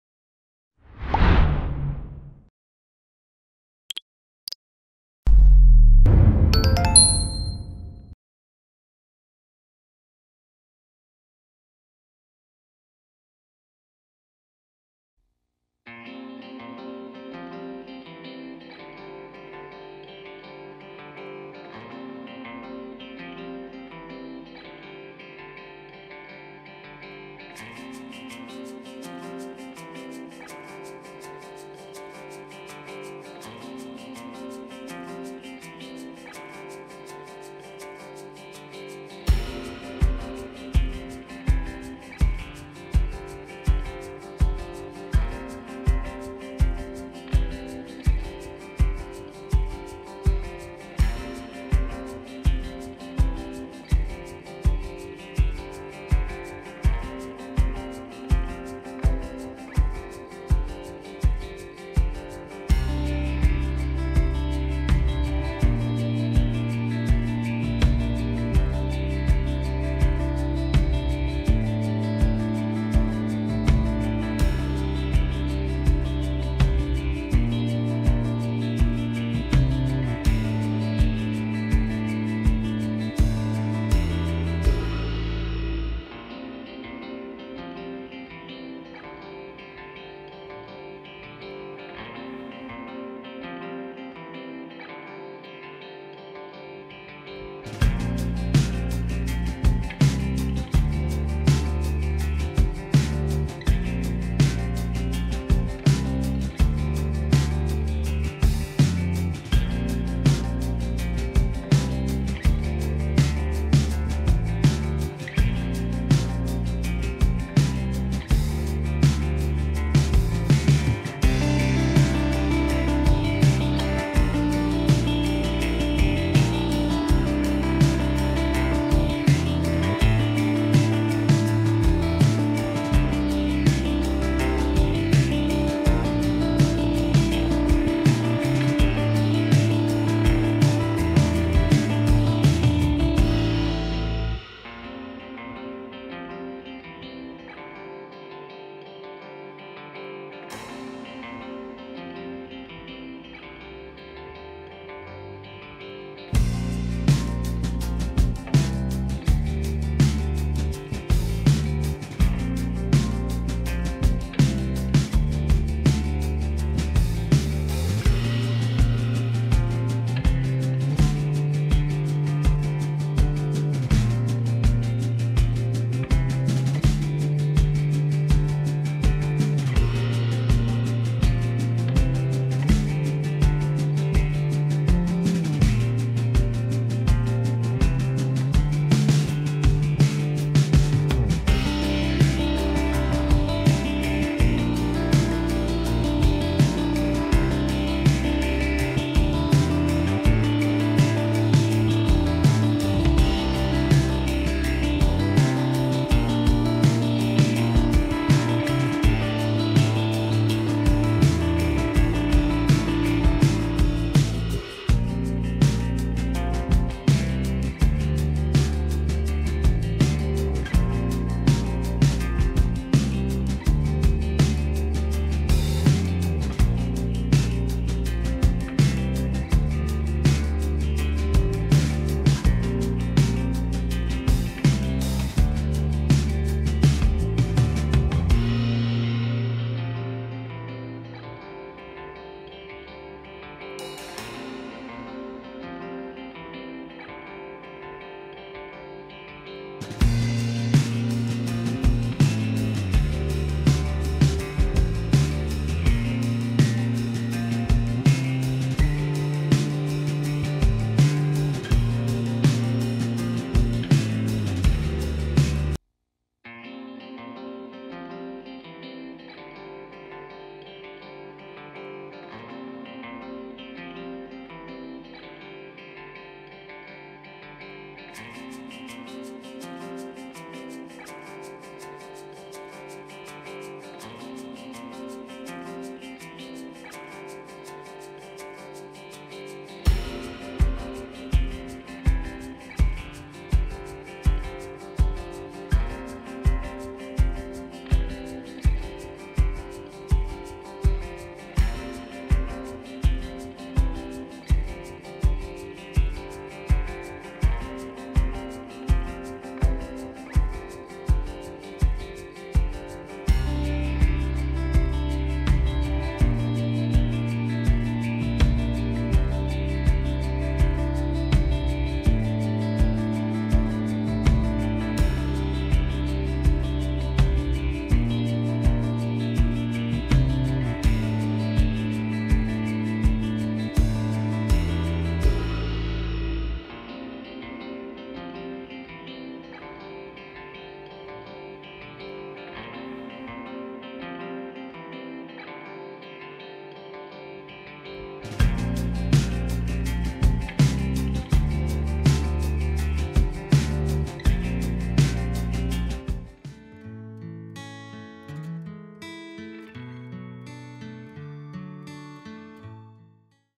Food Test - Milk Adulteration | DIY Science | ThinkTac
All kinds of theories, some true some outlandish, exist about the purity of the milk we buy and drink. Milk is often adulterated with all kinds of ingredients and/or chemicals. How amazing would it be if you could test the milk you drink and ascertain for yourself whether it is adulterated or not? That's exactly what we do here, through a series of tests.